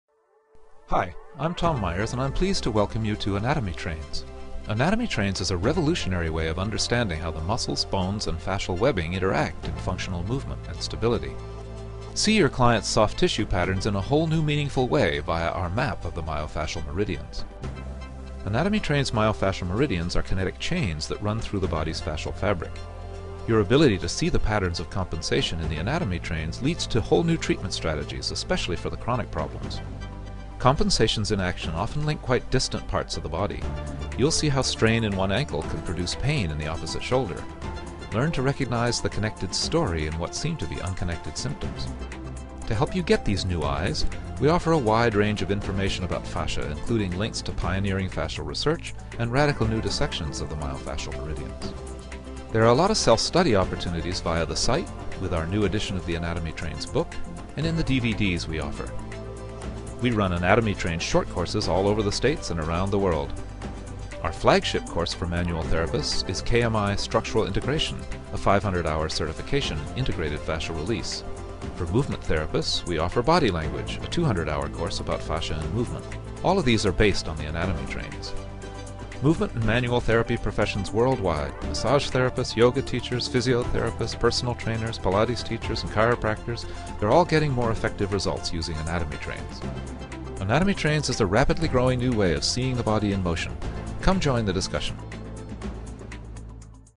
Anatomy Trains introduction
Get connected! An intro to connected myofascial anatomy and the Anatomy Trains concept.